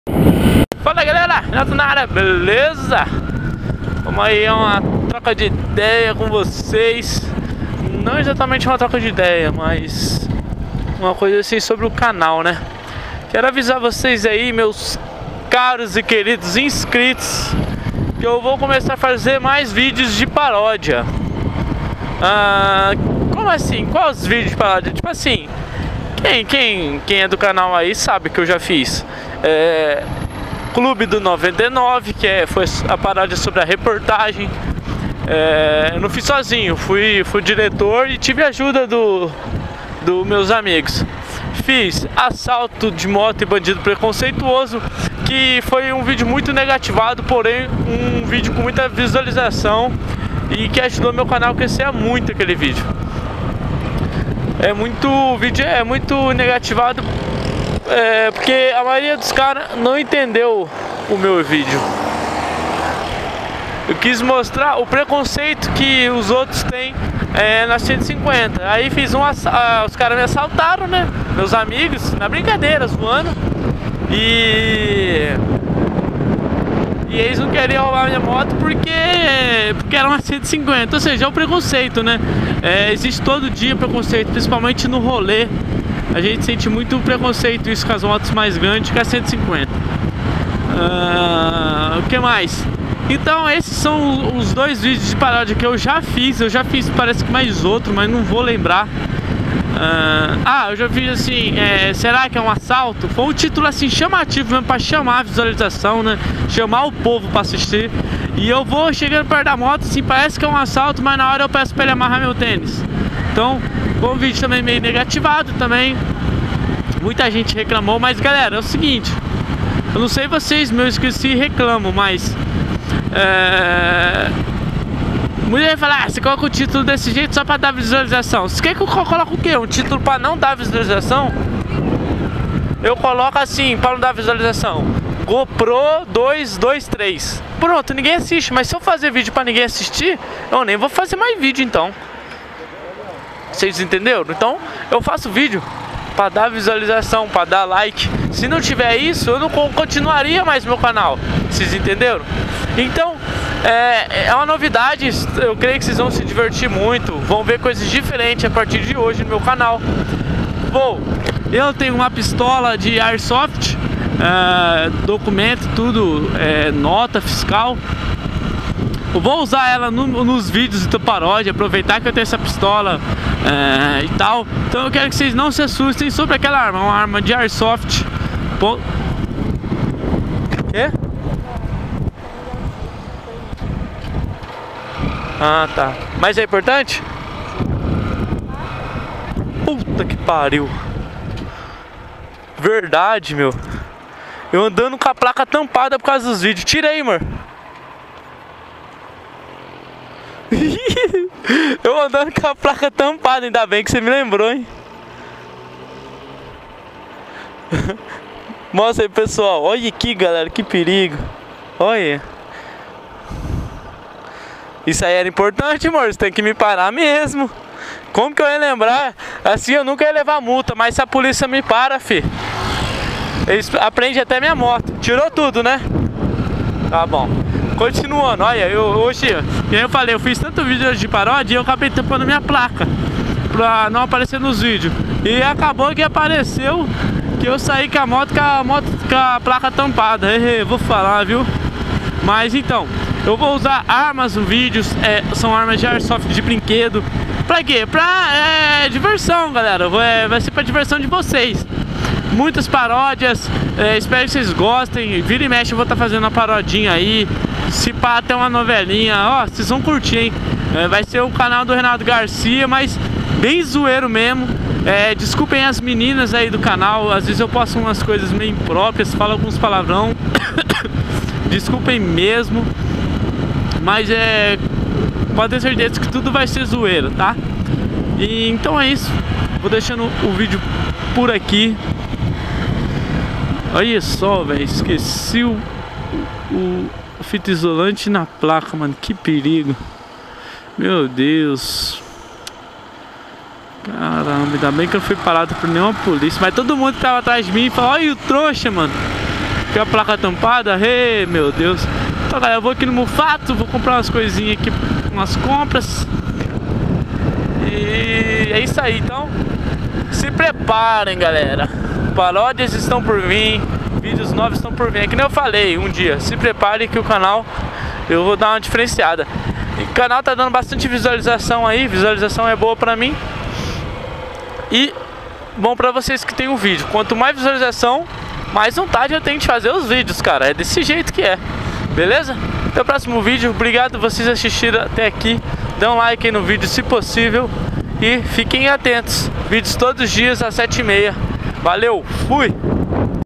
FAN 150 2014 BLACK- Se Preparem + Videos Paródias no Canal !!!
Fala galera !! Nesse video falo sobre o investimento em vídeos paródias para o canal , vídeos de tudo que é jeito !!!
Vamos fazer vários vídeos no mes , espero que curtem !!!
Sempre dando uma diferenciada !!! É NOIS ..
Falei que haveria coisas novas para o canal ,e ai está uma !!
É NOISES !! De preferencia , sempre vou tentar deixar os vídeos paródias para os dias de DOMINGO ...
ABRAÇOSS LIKE PRA AJUDAR !!!
Moto : FAN 150cc 2014 PRETA ESDI
Câmera : Gopro Hero 2
Cidade : Londrina - Paraná
Motovlog Londrina